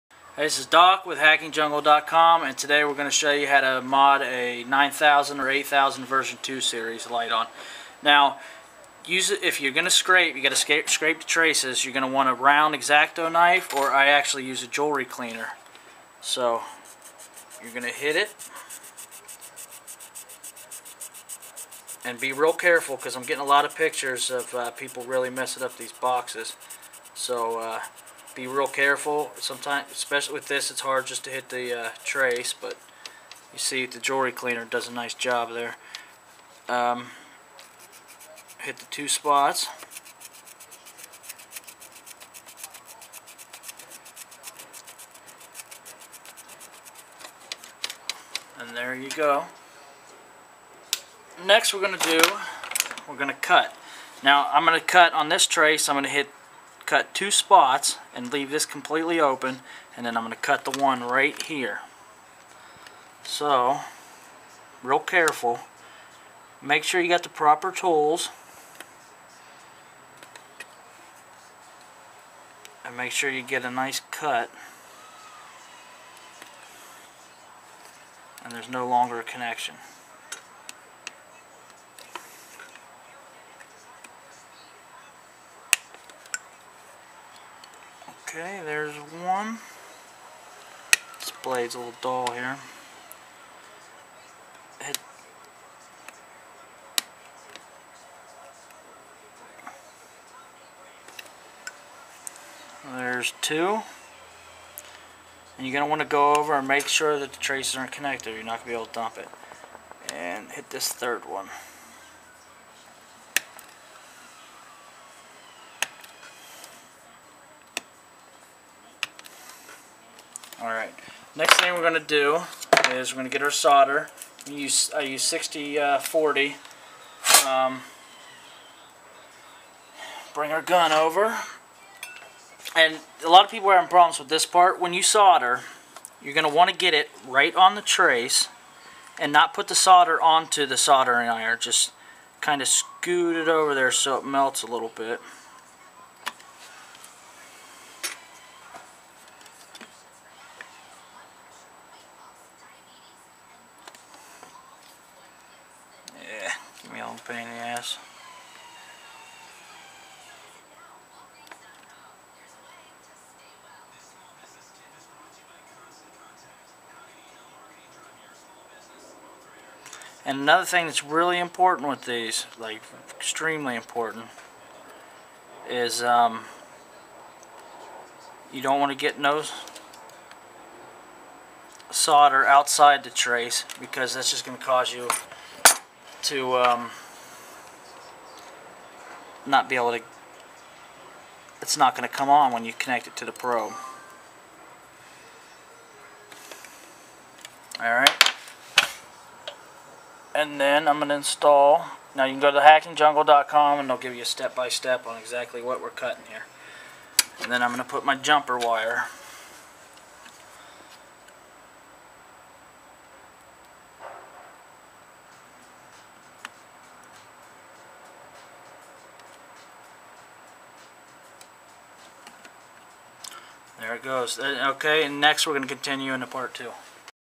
Hackingjungle.com presents Flashing a Lite on 83850 V2 or 93450 JungleFlasher 1.73 Part 1 of 4
Hackingjungle.com presents Flashing a Lite on 83850 V2 or 93450 JungleFlasher 1.73